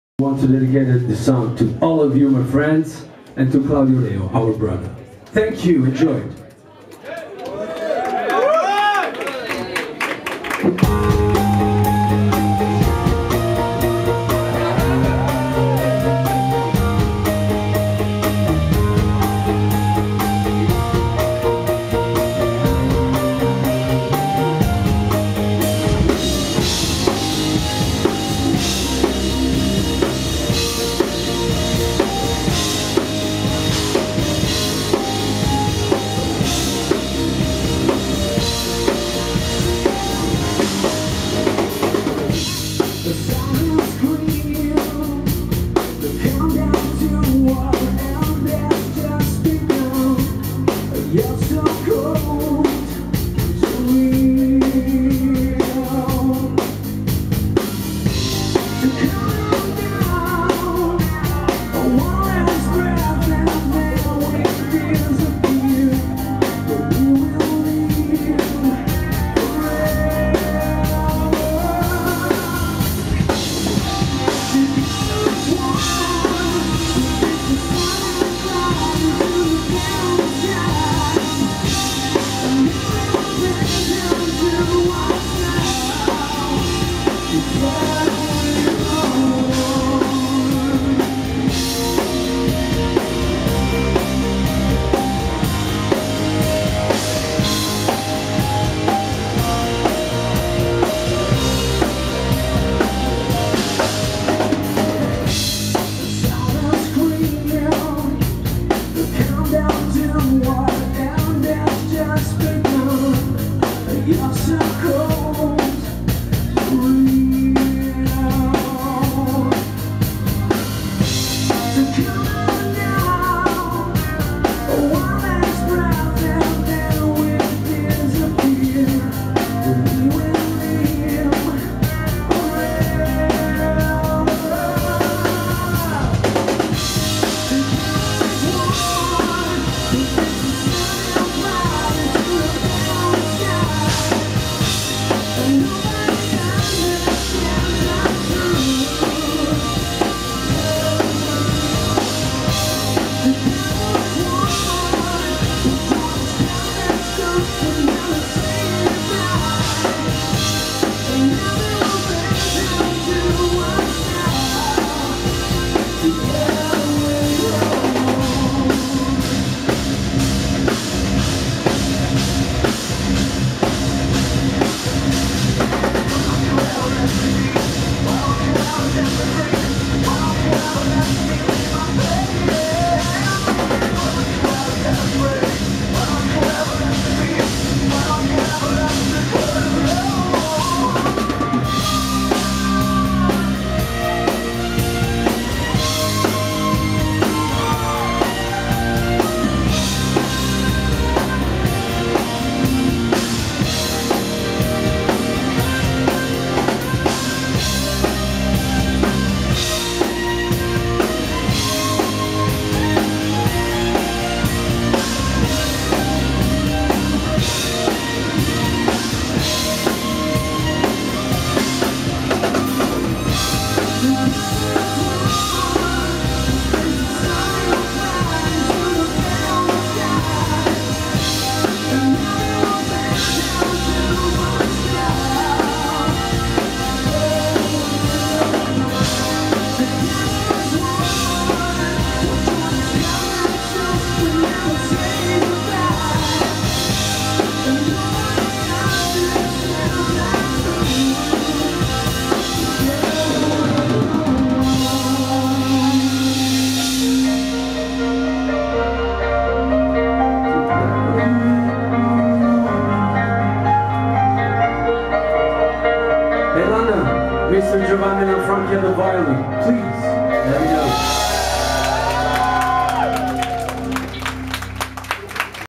Cayne Live @ The Borderline 14/3/13
Cayne from Itay second support comming on after The Great Marlarkey With tmain band Viza.Three very good bands in one night. Brought a cd fom both supports acts.Cayne have a new CD out now well worth a listern.